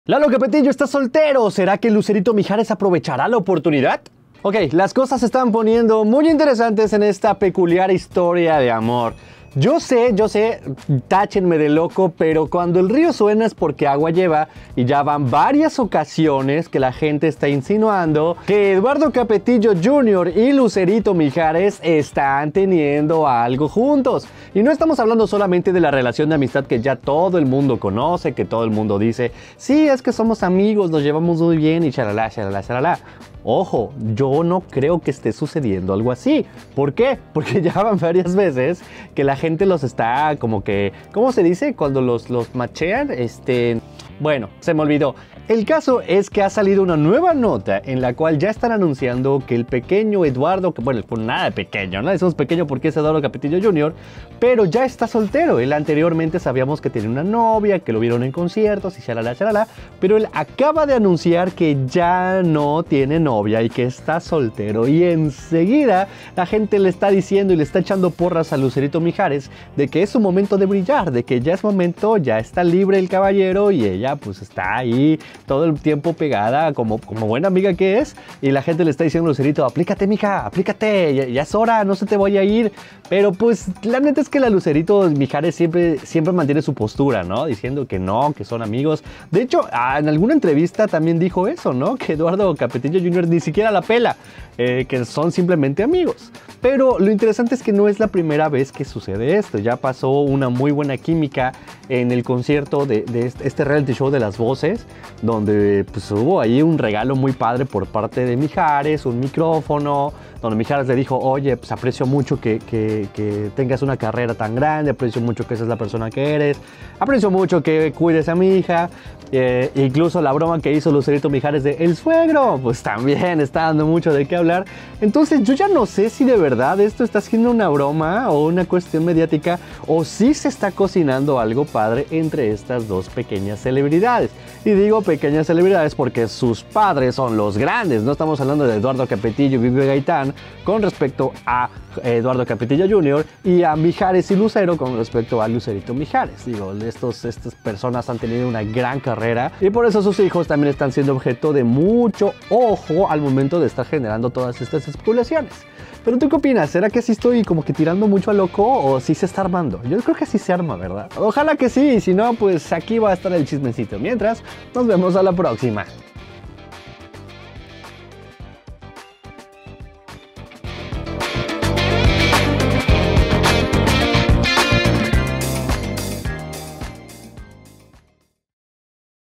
Lalo Capetillo esta soltero,¿Lucerito Mijares aprovechará la oportunidad?
El cantante LaloCapetillo asegura en una entrevista que se encuentra soltero,¿LuceritoMijares aprovechará la oportunidad?